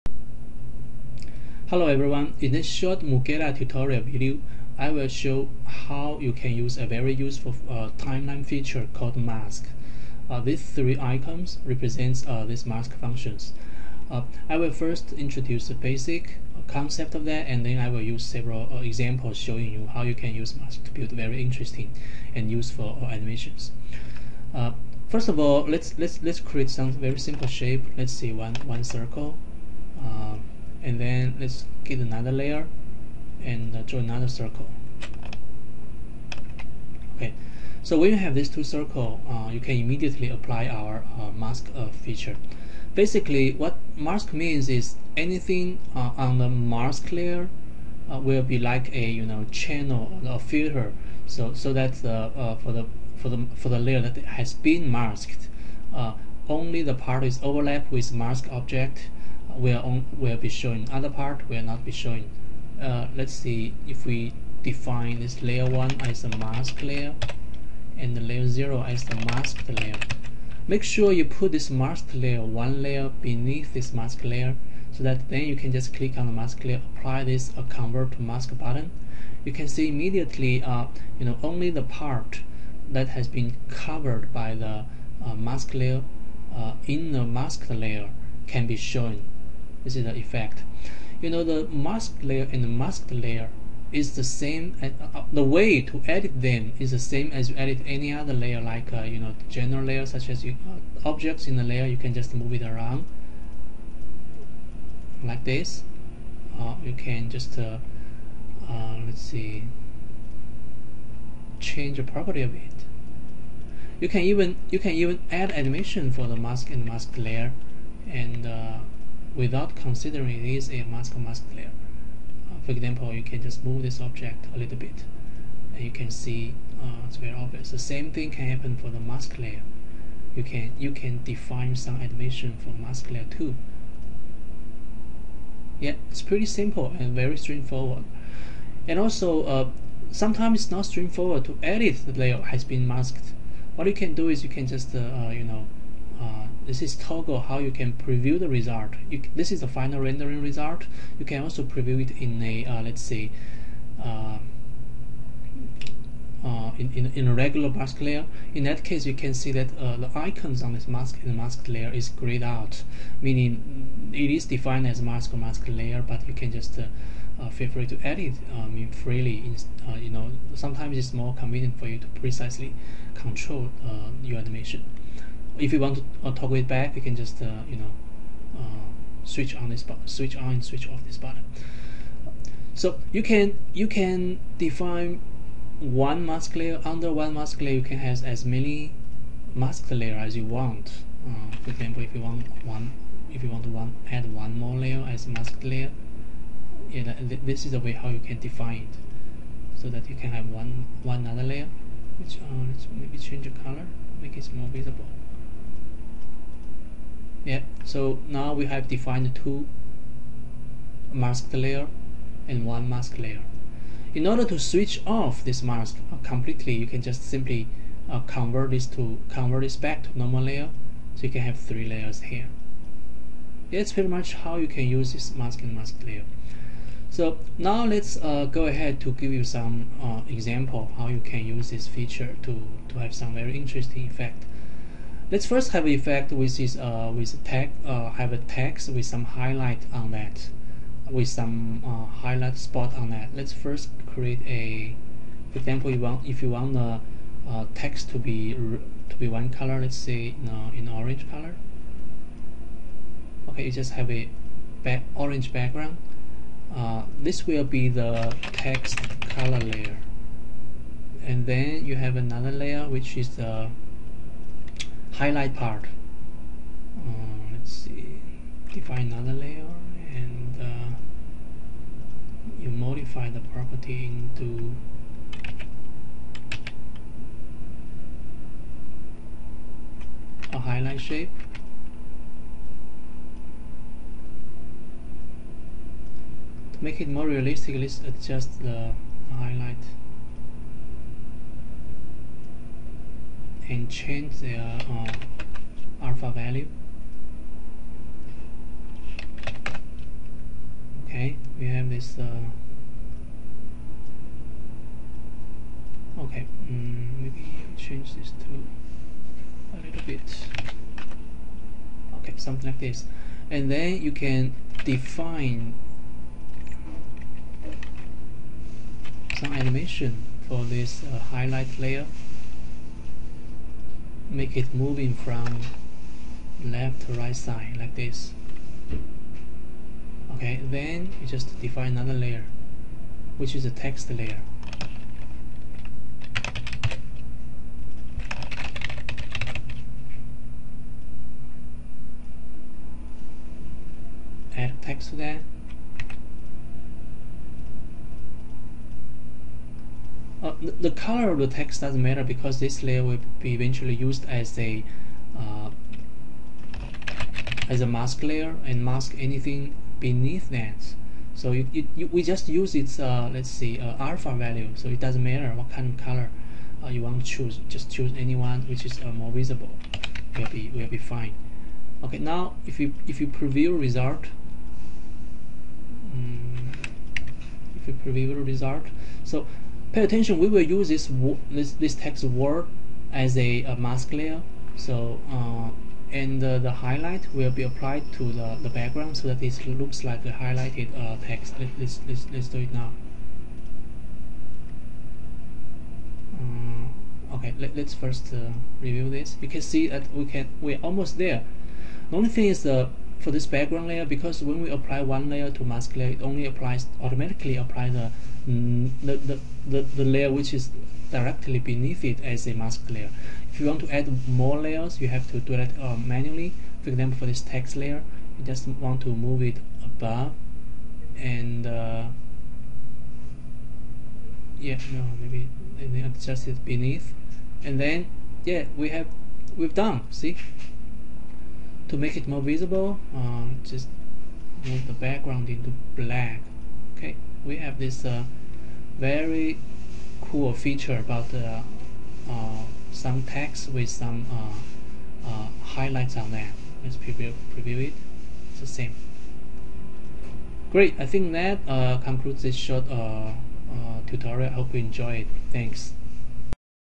Mugeda: Mask Animation
This short Mugeda tutorial shows how to use Mugeda's mask feature to achieve cool special effect.